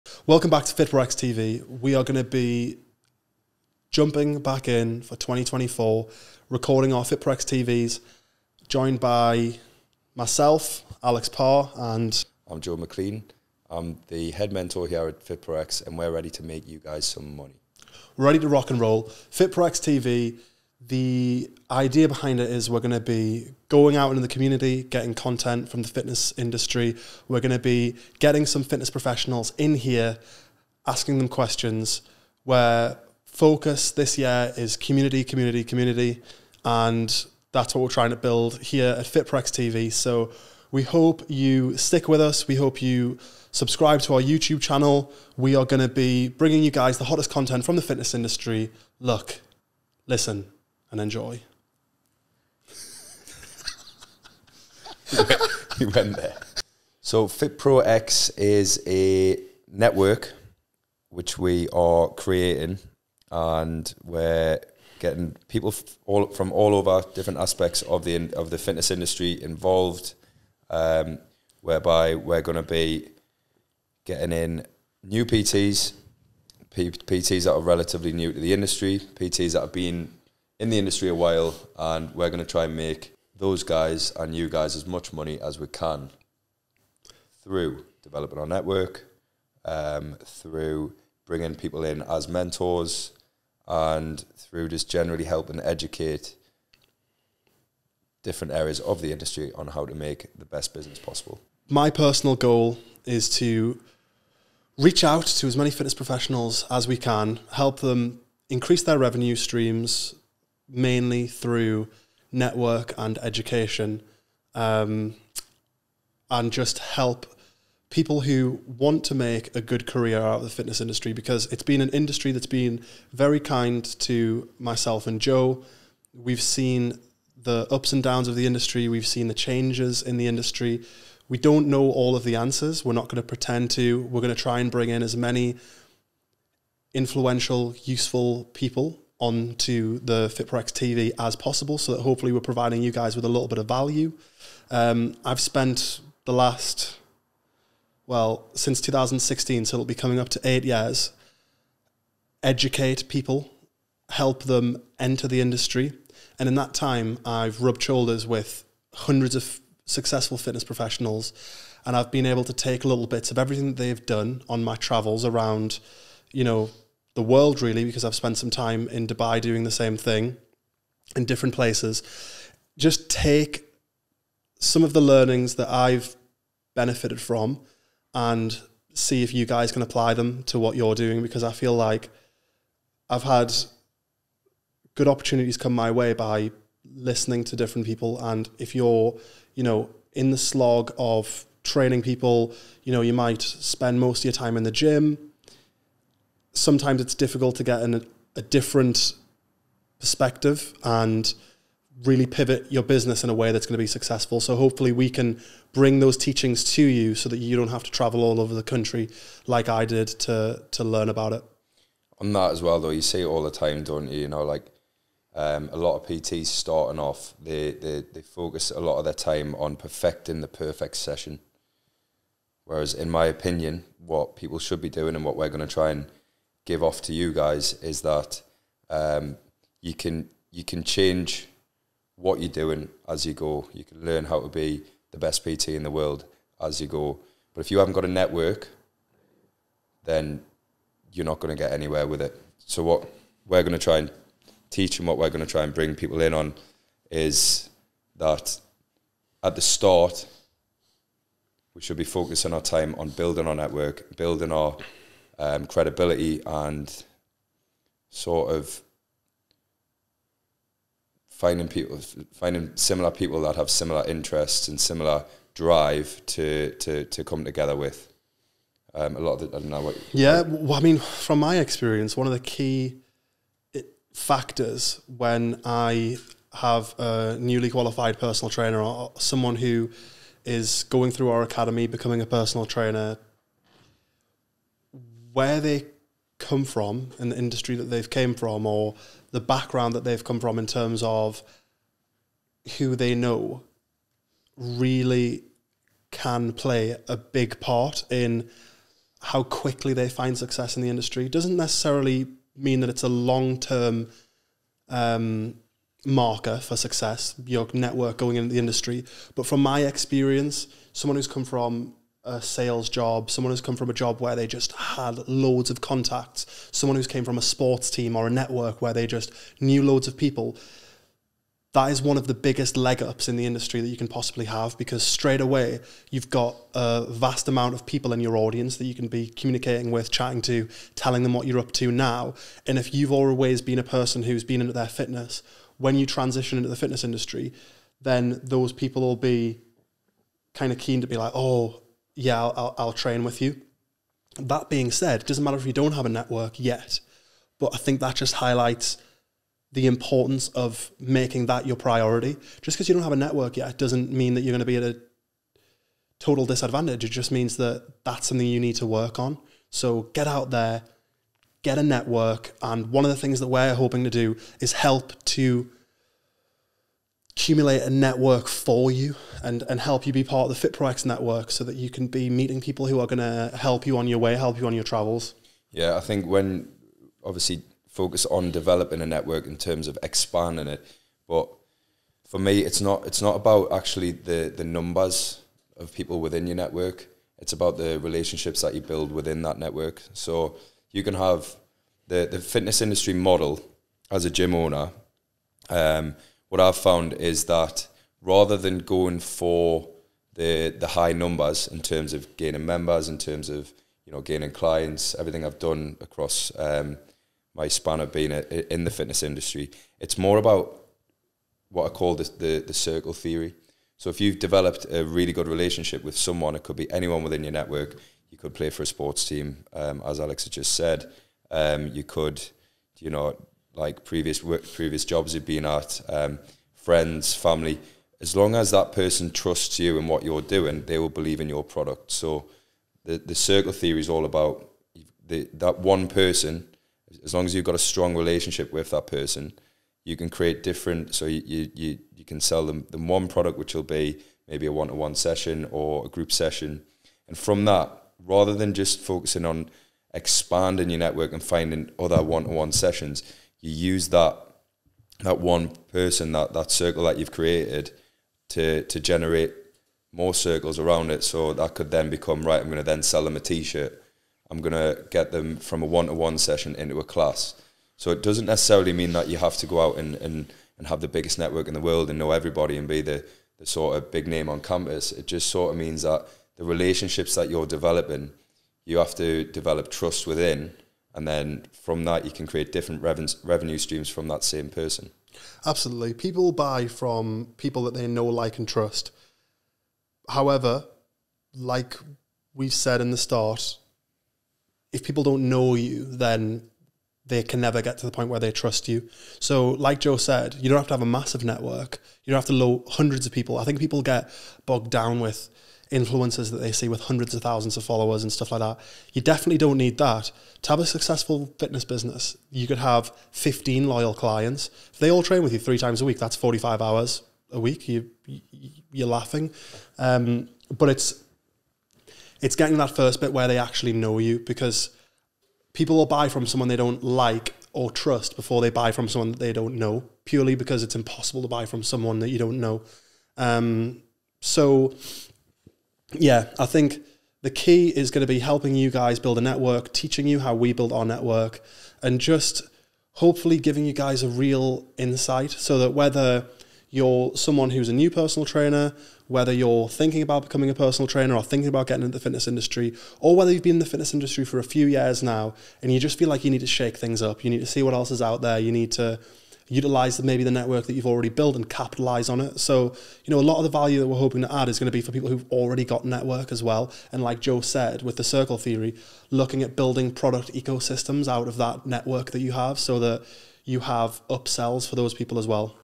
FITPRO-X TV - Combining Fitness with Education.
🎙️ We are back & ready to bring you guys another fresh episode of FPX TV to kickstart 2024! In the hot seat this episode we have our host Alexander Parr & Joe McLean owner of BodyHacks Gym. 🏋🏼

🎧 Tune in to hear all about mentoring here at FPX & what we want to bring for 2024...